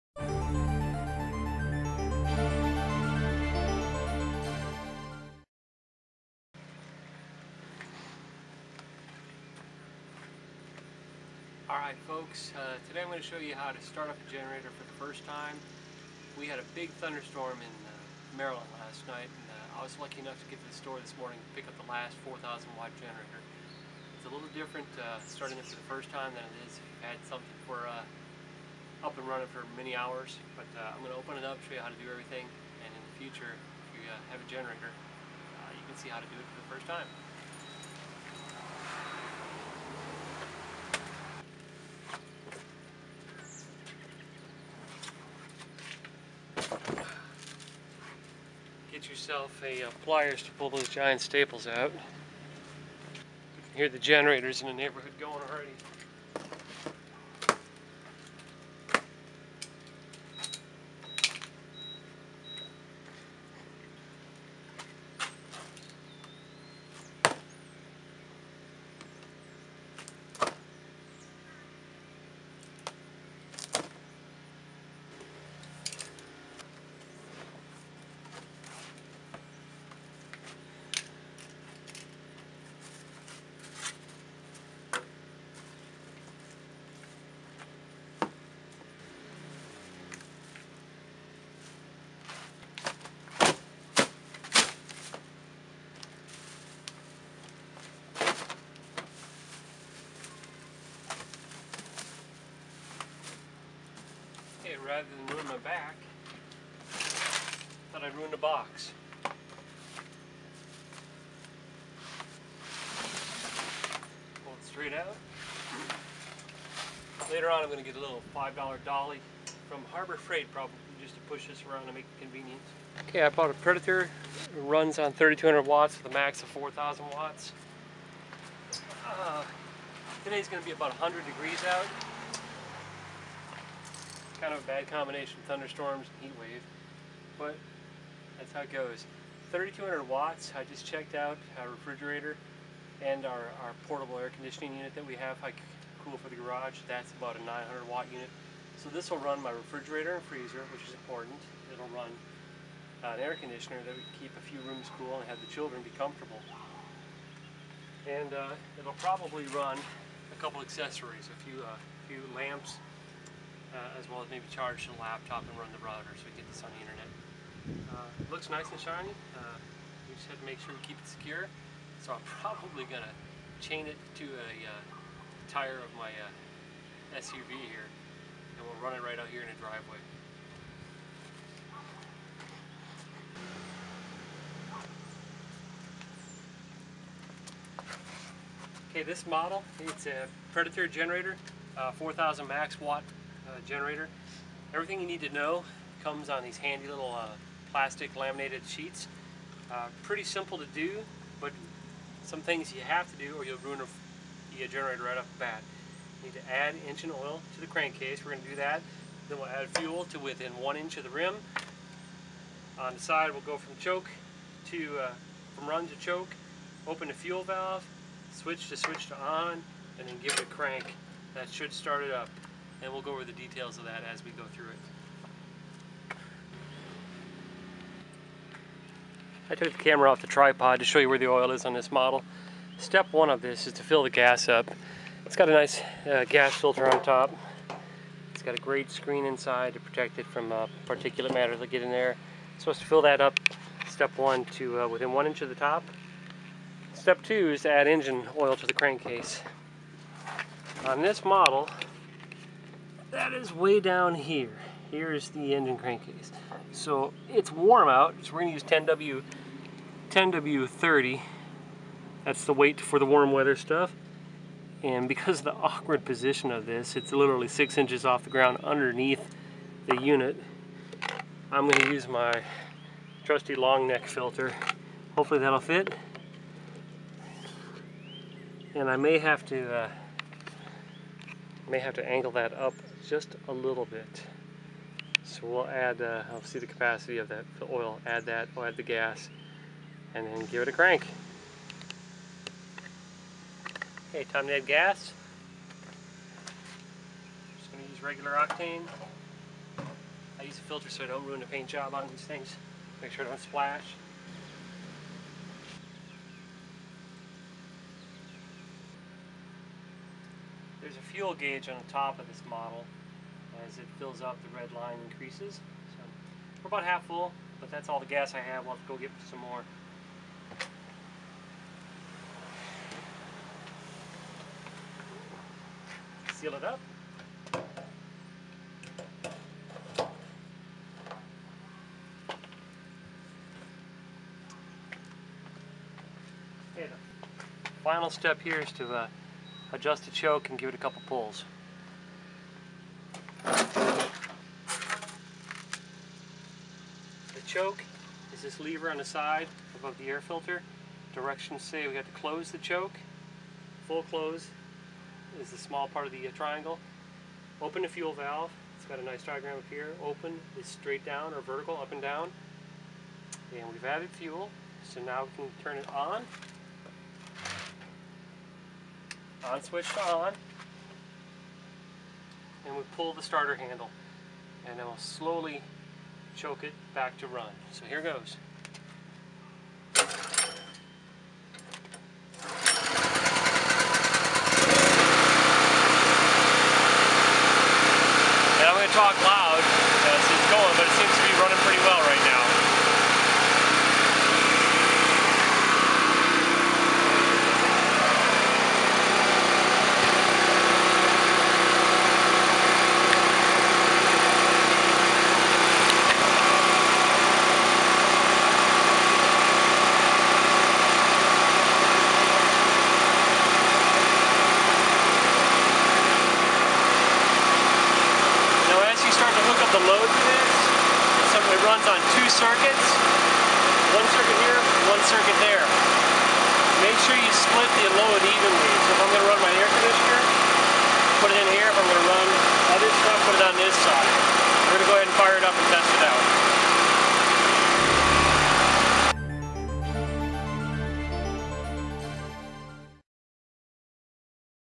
How To Set Up an Electrical Generator
Step by step guide to setting up and starting your Generator for the first time.